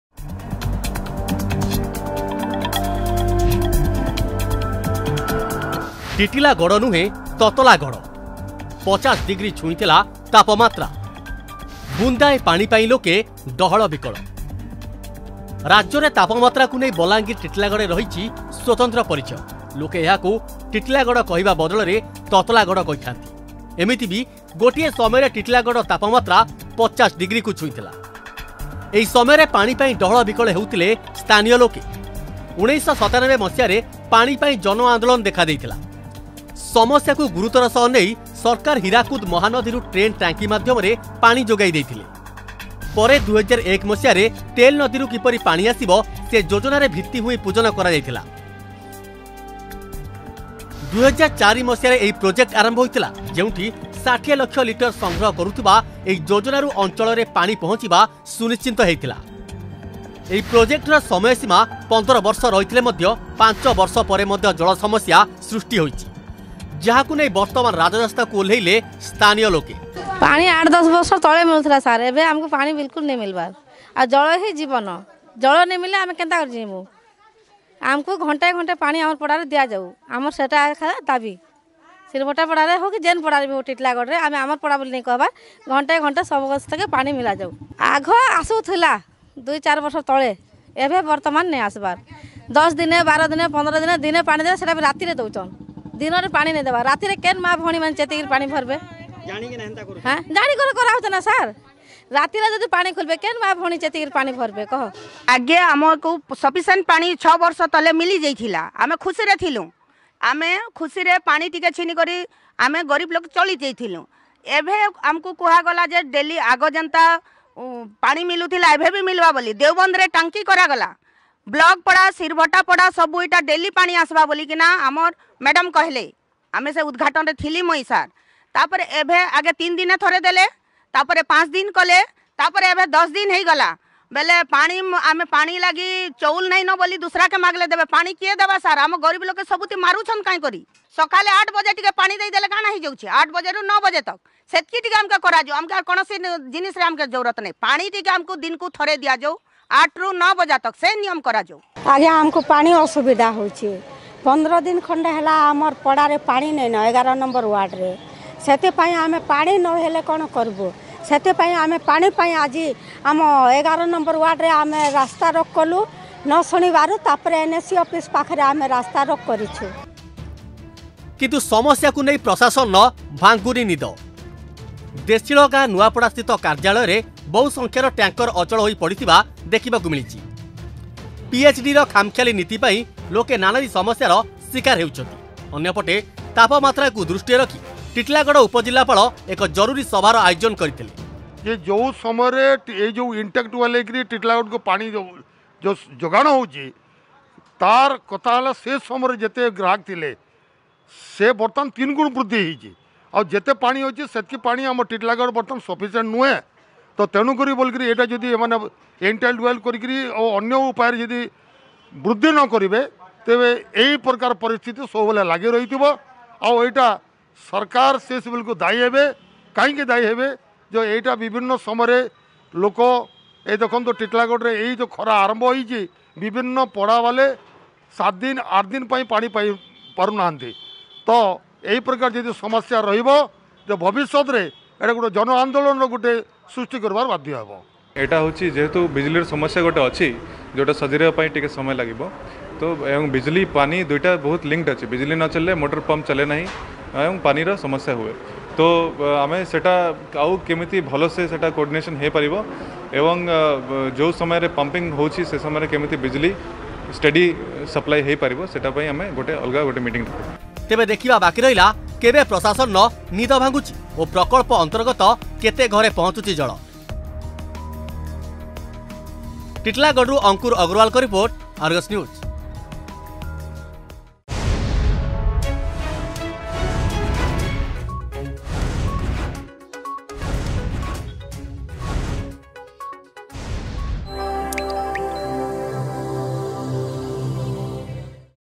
ବୁନ୍ଦାଏ ପାଣି ପାଇଁ ଲୋକେ ଡହଳ ବିକଳ
ରାଜ୍ୟରେ ଅସହ୍ୟ ଗରମ କଥା ସାମ୍ନାକୁ ଆସିଲେ ବଲାଙ୍ଗୀରର ଟିଟିଲାଗଡ଼ ସଙ୍ଗେ ସଙ୍ଗେ ମନେ ପଡ଼ିଥାଏ । କେତେକ ଲୋକେ ଏହାକୁ ଟିଟିଲାଗଡ଼ ନୁହେଁ, "ତତଲାଗଡ଼" କହିଥାନ୍ତି । କିନ୍ତୁ, ଟିଟଲାଗଡ଼ରେ କେବଳ ଅସହ୍ୟ ଗରମ ସହ ଜଳ ସଙ୍କଟ ମଧ୍ୟ ଦେଖିବାକୁ ମିଳିଛି । ଯାହାକୁ ନେଇ ବର୍ତ୍ତମାନ ରାଜରାସ୍ତାରେ ଲୋକେ.